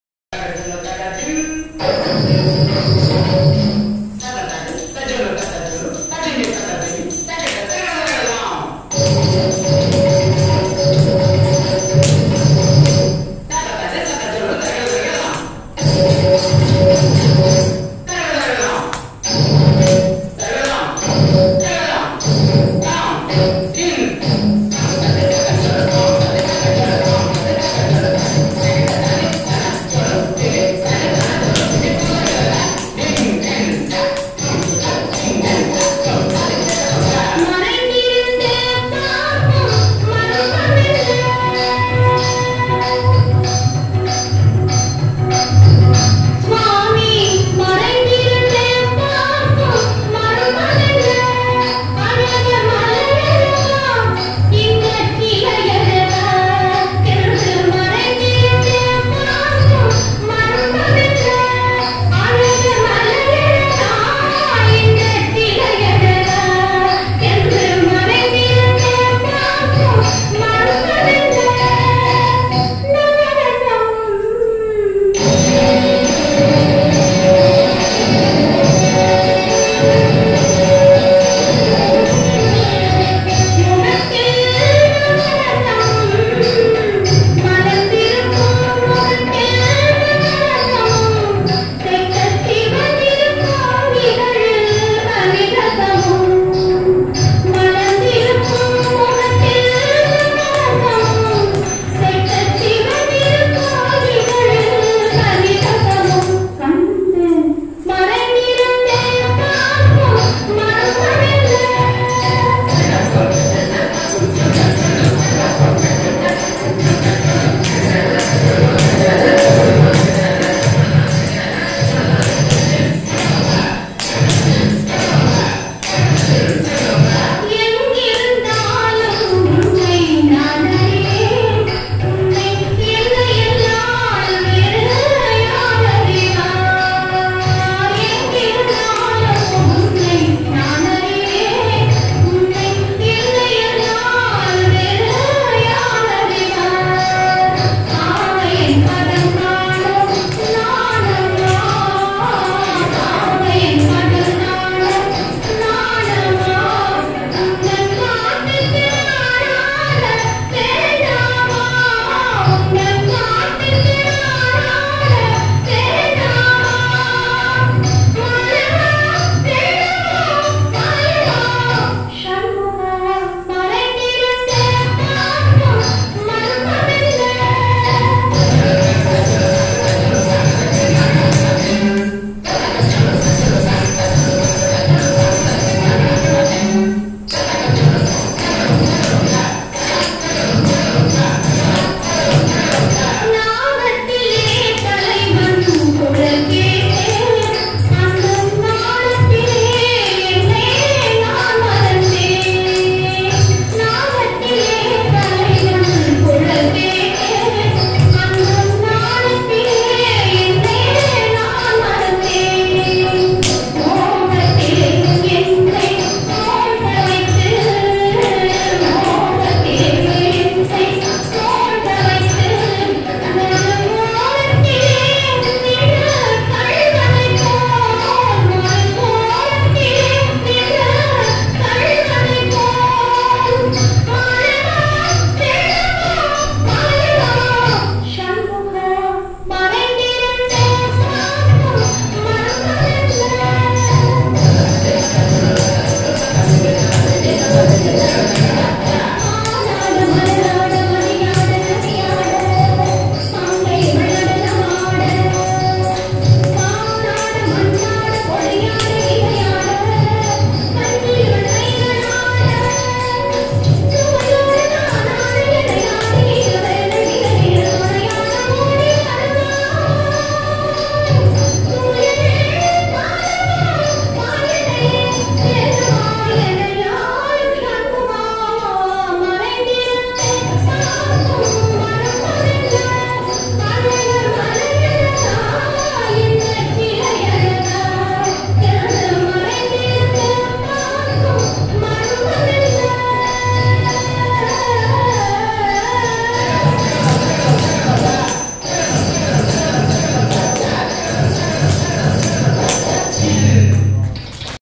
பரத நாட்டியம் நெய்வேலி கலாலயம் 28 November 2023
செம்பியன் திருமேனி நூல் ஆய்வரங்கம் - ஆசிரியர் சிங்கை மா.அன்பழகன். 28-11-23- தாய் ராசி ஜெகதீஸ்வரன் ஏற்பாடு. படச்சுருள் பண்ருட்டி முனைவர் கவிதைகணேசன்.